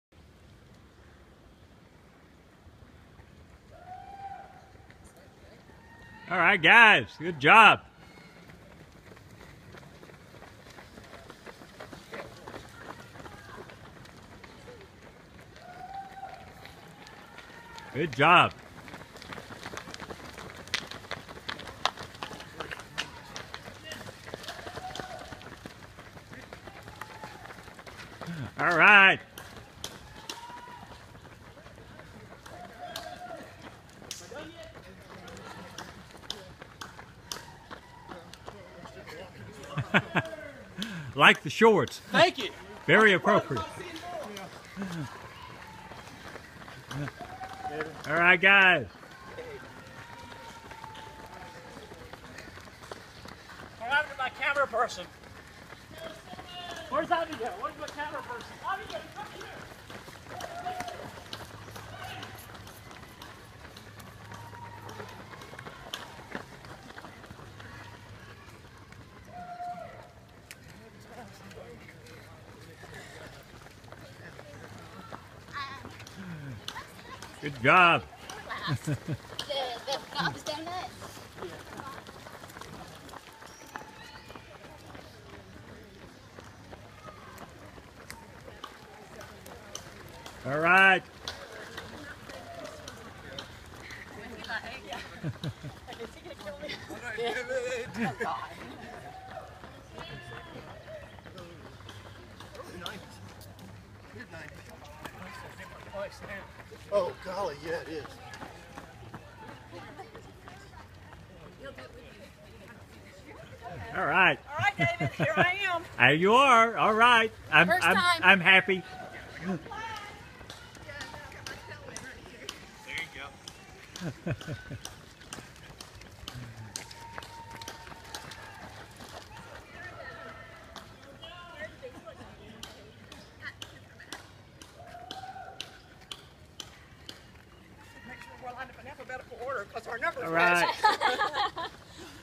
Start of the Memorial Day Weekend Trail Race
Memorial Day Weekend 6 & 12 Mile Trail Race by Southeastern Trail Runs. The start.